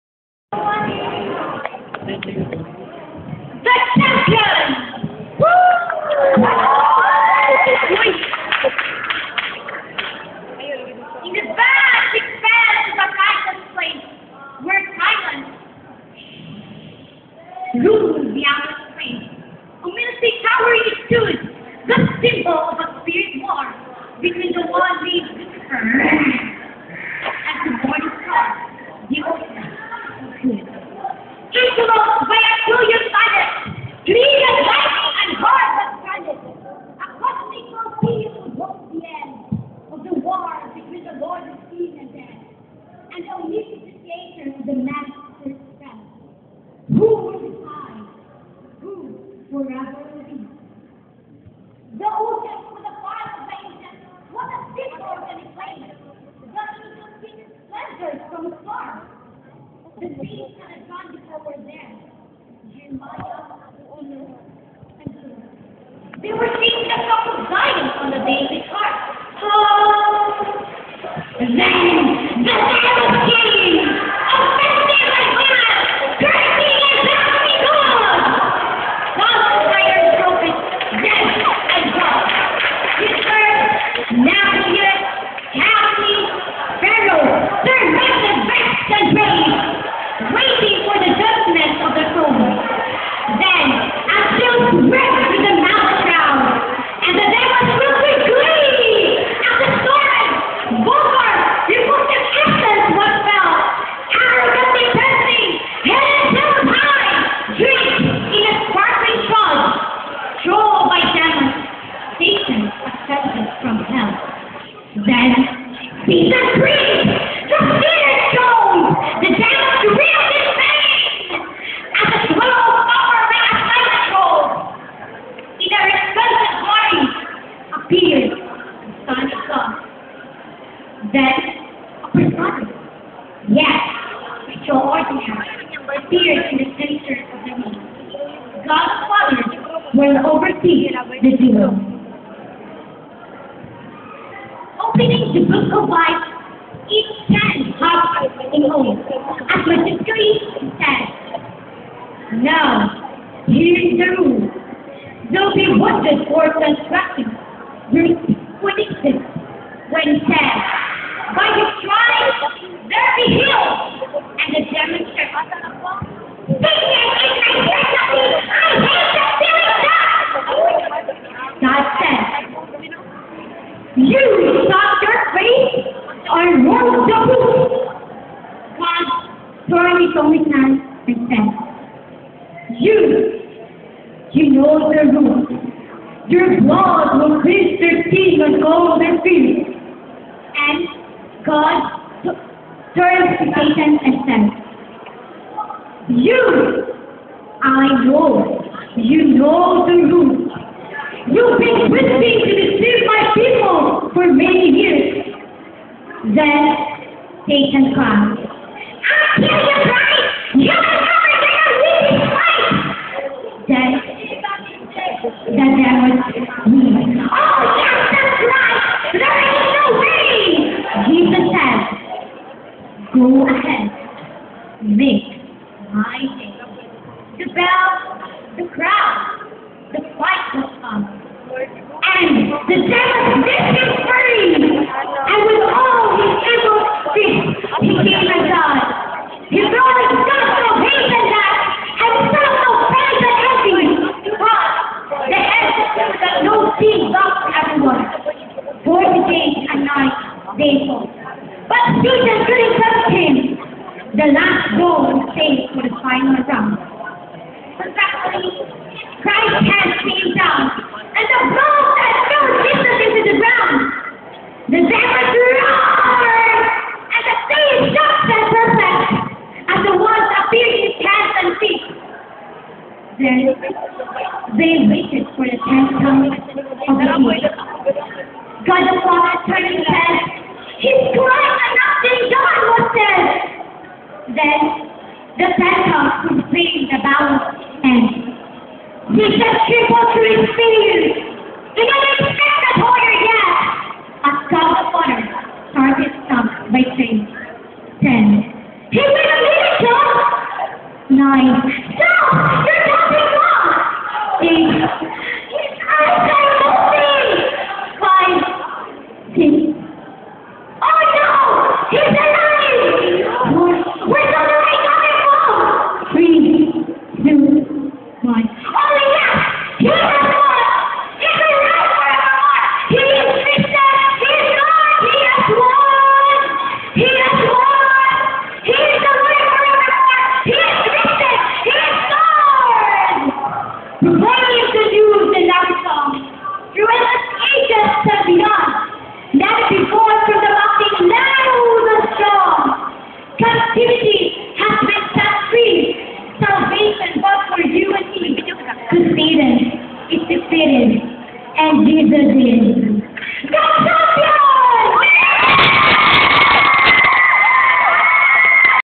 The Champion (Declamation -- Rose Christel Wahing)
University of the Visayas
High School Department
English Month 2011
The Seniors
Declamation